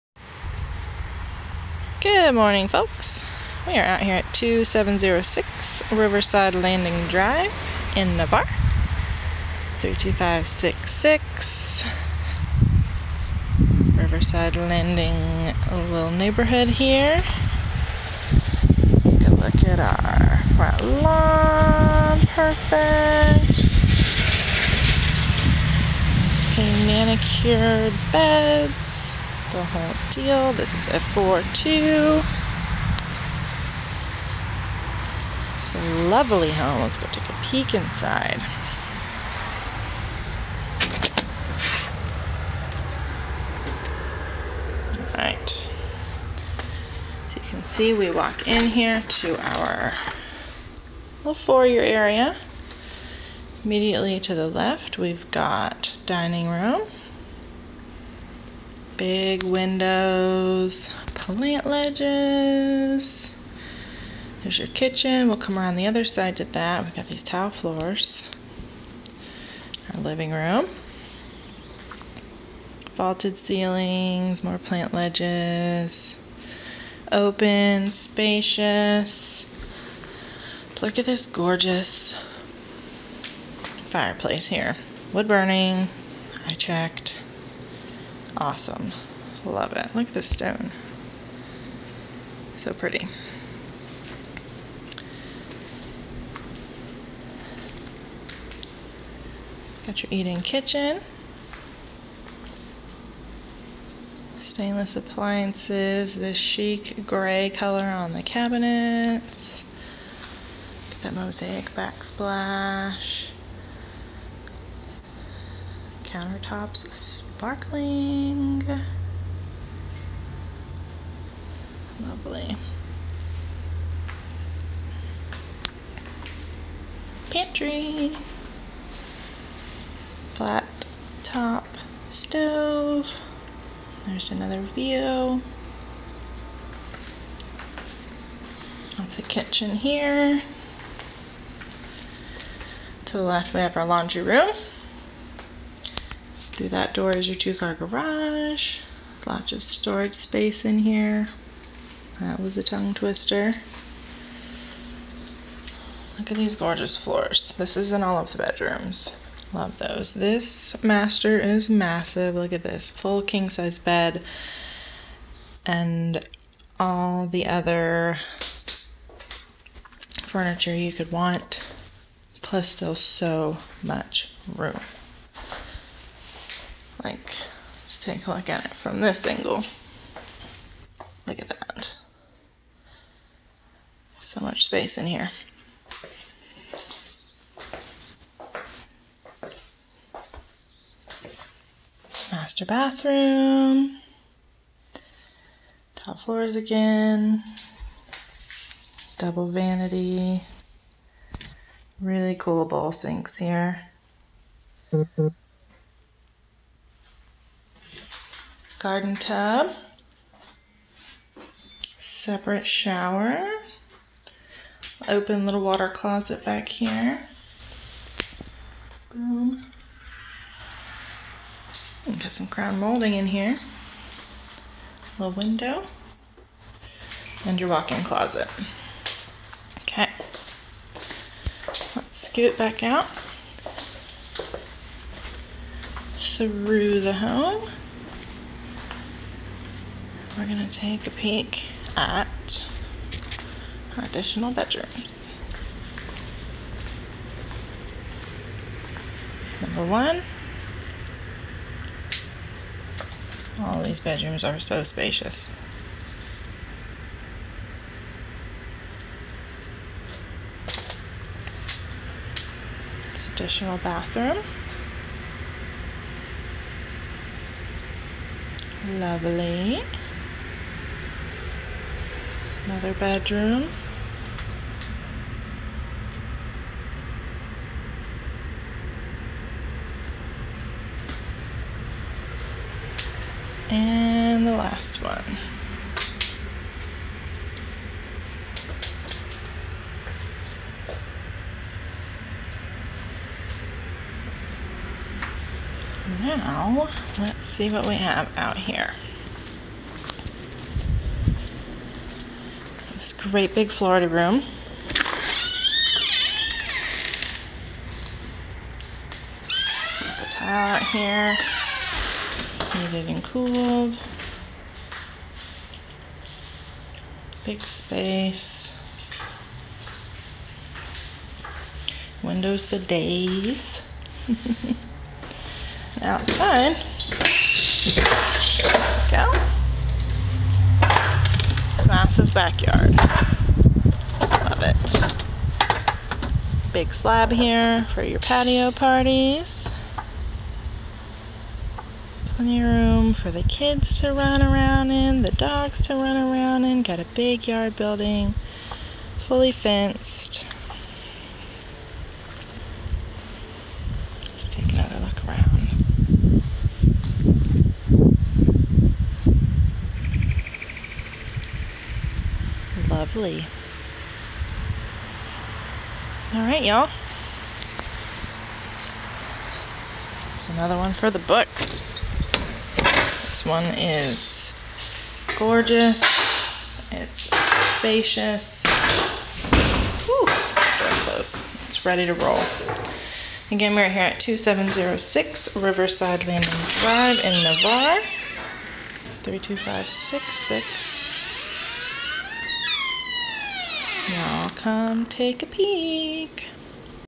Navarre Homes for Sale | 2706 Riverside Landing Dr, Navarre, FL 32566
If Navarre living is your jam, then come on out to Riverside Landing and check out this 4/2.  When you arrive you will notice the charming landscaping that accompanies you up to the front door.  Once inside you are greeted by a bright formal dining room immediately to your left, and straight ahead you will find the large, open living room.  Vaulted ceilings and a white and grey stone fireplace really bring this space to life, not to mention the french doors that open to the Florida room, letting in even more natural light.  Around to the left is the kitchen and breakfast nook. Quartz countertops shimmer with every step you take, and go nicely along with the mosaic backsplash and stainless steel appliances. On this side of the home you will also find an ample master suite. Space enough for your king size bed with plenty more room to move around comfortably.  The master bath boasts a double vanity with beautiful and unique bowl sinks, a garden tub,separate shower and walk in closet with built in storage.  Across the home we find three additional bedrooms, each with a rich, wood-laminate flooring (that you will also find in the master), and another full bathroom. The Florida room we spoke of before, just off of the living room, is host to wall to wall windows that give you a full view of the backyard.  The backyard itself is just as spacious as the rooms inside. Plenty of lawn for your kids or pets to frolic!  The grass is zoysia, and easily upkept with the sprinkler system (new, as of 2017).  The yard building was built in 2014 and includes a double sided loft and a 40 year transferable warranty on the steel roof.